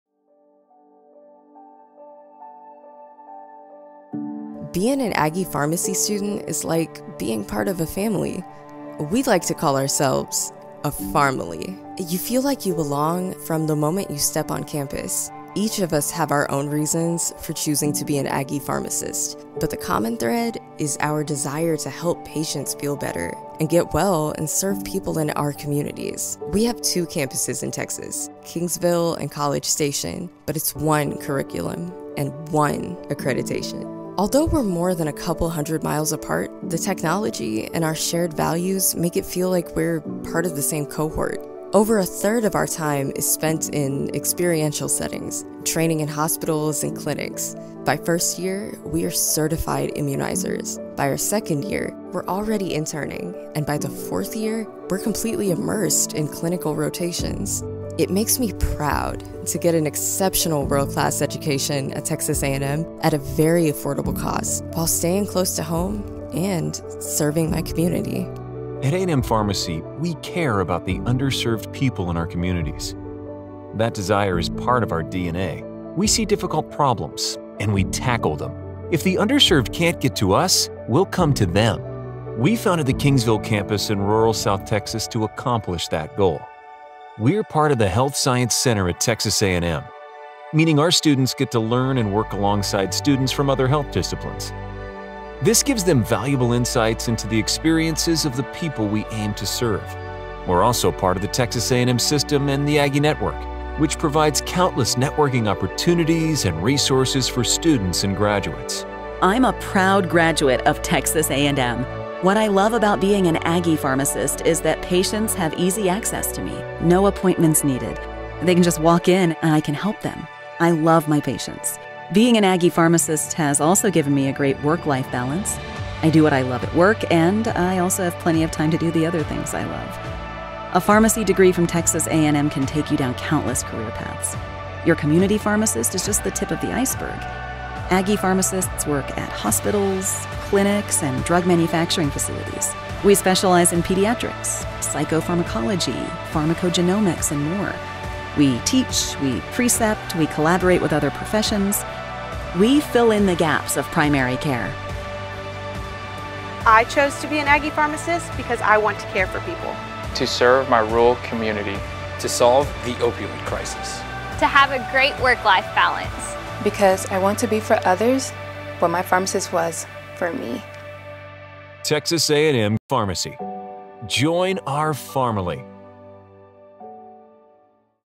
About the Texas A&M College of Pharmacy
Explore both of our campuses, hear from our students, and get a better understanding of what it means to be an Aggie Pharmacist!